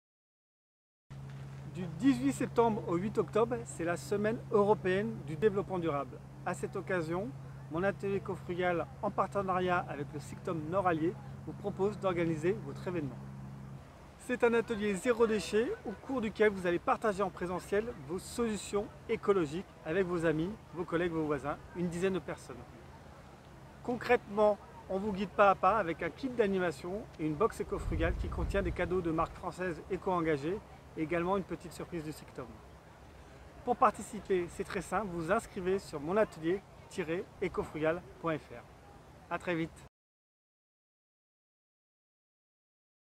Atelier Ecofrugal - Inscrivez-vous !
A l'occasion de la SEDD 2020, le SICTOM Nord Allier vous invite à devenir Ambassadrice/Ambassadeur, le temps de l'Atelier Ecofrugal de la thématique de votre choix. Des cadeaux pour tous les participants...